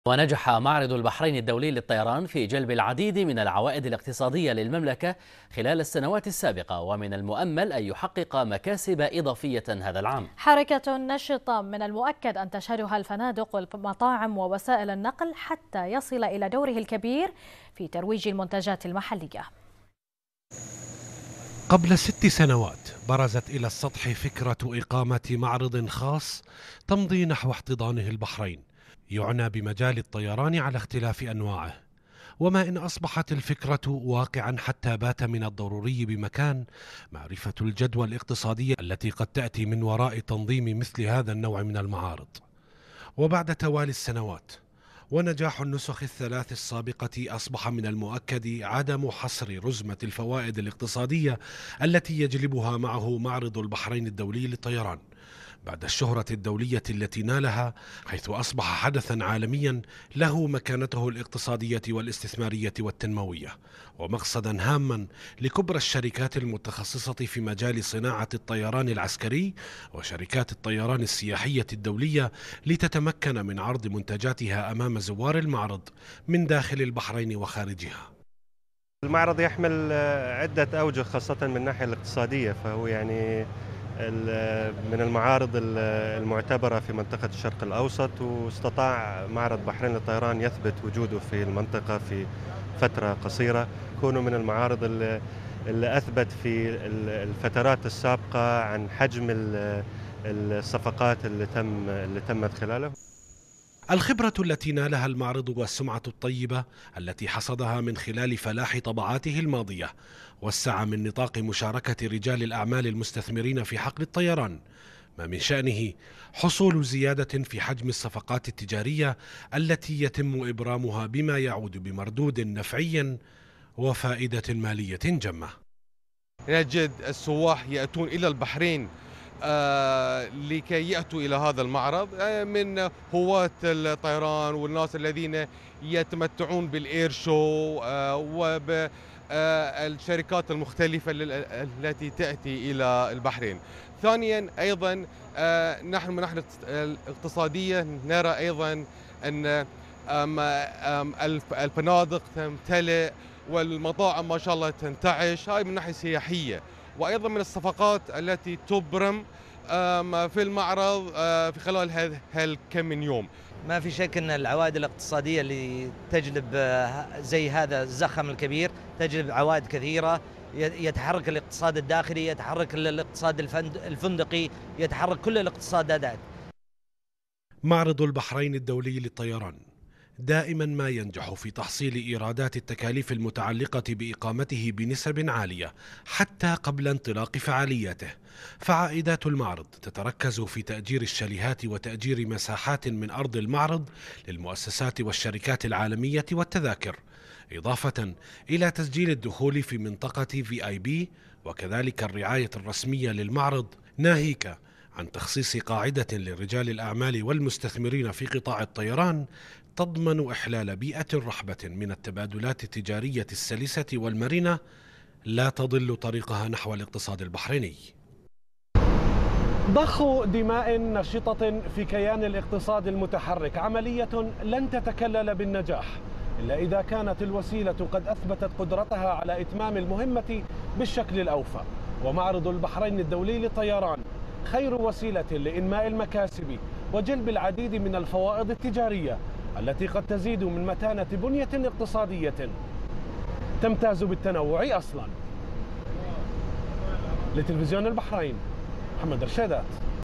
البحرين : مكاسب تجارية يتكفل بتحقيقها معرض البحرين الدولي للطيران
البحرين : مكاسب تجارية يتكفل بتحقيقها معرض البحرين الدولي للطيران بما يعود بالنفع على الاقتصاد الوطني